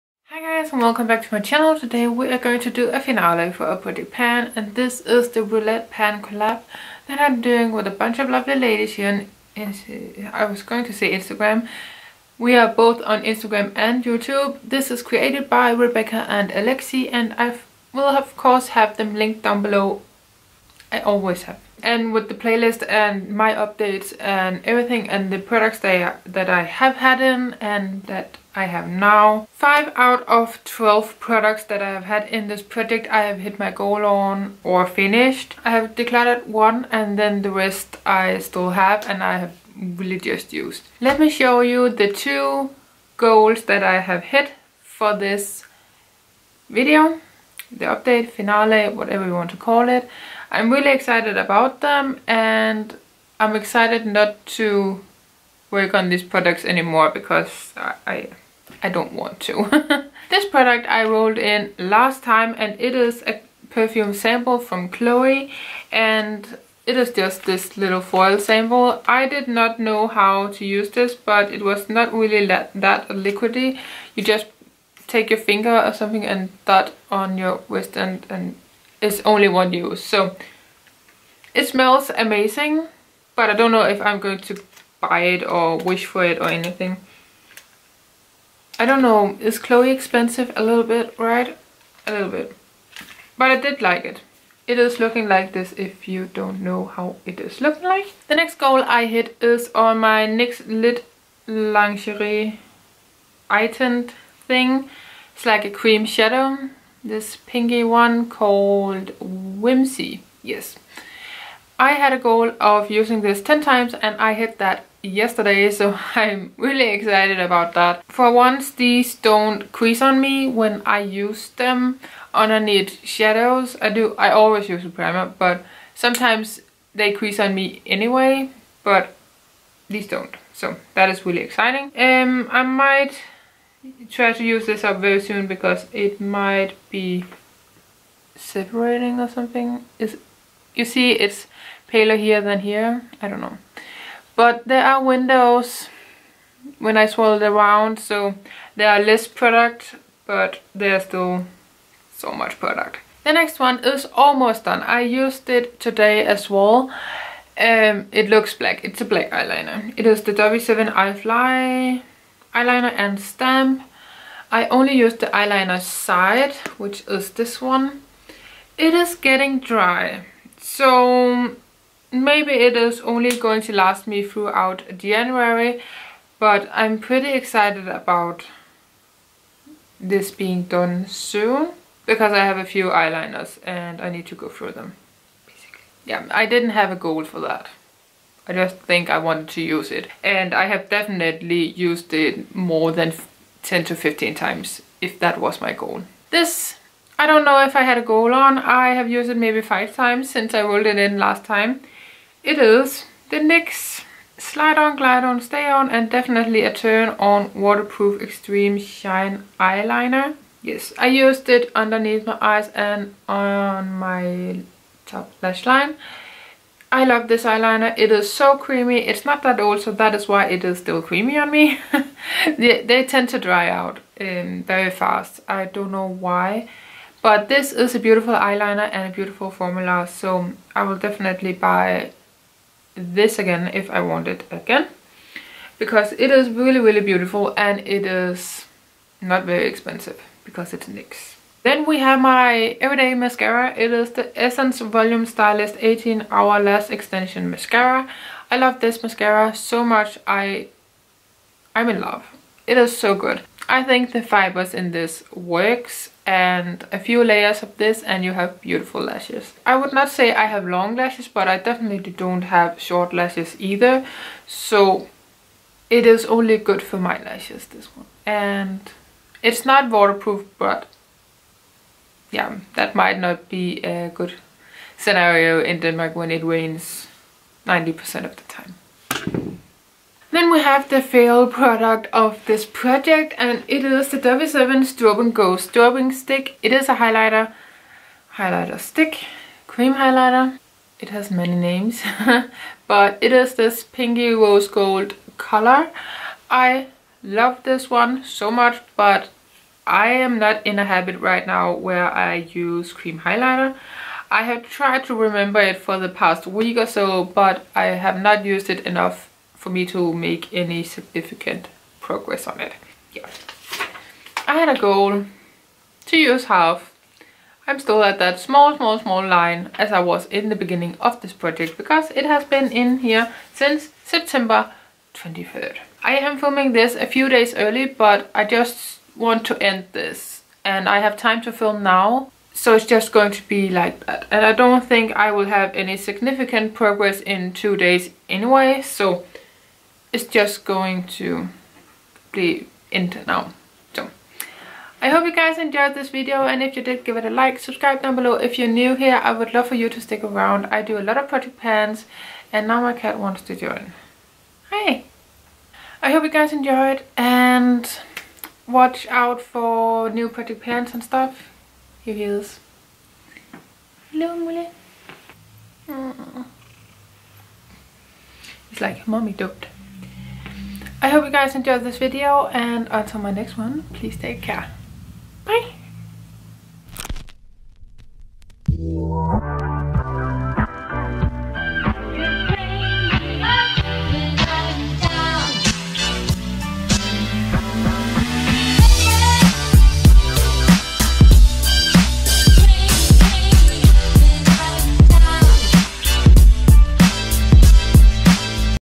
ROULETTE PAN COLLAB | FINALE // ROUND 6
It's time for MY second round of the roulettepancollab that Rebecca and Alexy created! This is the 6th round .

This round starts on the 23rd of september and will run until the 23rd of january.
There is a a list of 50 categories, where you randomly select 6 of thecategories and then pick 6 products corresponding to these categories.
When you finish or hit goal on an item you have the option to roll another product in!

On Instagram you can find everyone on the hashtag RoulettePanCollab

PRODUCTS:

6)  An old favorite (DECLUTTERED)
 • Maybeline Color Tattoo - Permanent Taupe
13)  A product you recieved as a gift (DONE)
 • Meraki Makeup Wipes
26) A matte Product (DONE)
 - W7 Go Consealer - Fair*
32) Something in your favorite shade
 - W7 Stobe & Go strobing stick - Pink light*
33) Something overrated (DONE)
 - Benefit Blush - Coralista
 1) A collab product (DONE)
 - BH Cosmetics Shaaanxo palette
 2) A hair care item (DONE)
 - Batiste Dark brown Dryshampoo
 8) A product of your choice
 - W7 Eye Fly eyeliner
 15) A product you haven't used much
 - Essence Volume Stylist lash extension mascara
 36) A product from Nyx (DONE)
 - Nyx Lingerie Lid Tint - Whimsy

 37) A product that smells nice (DONE)
 - Cloé perfume sample
 39) A colorfull eyeliner
 - NYX Glide on pencil - Pretty Violet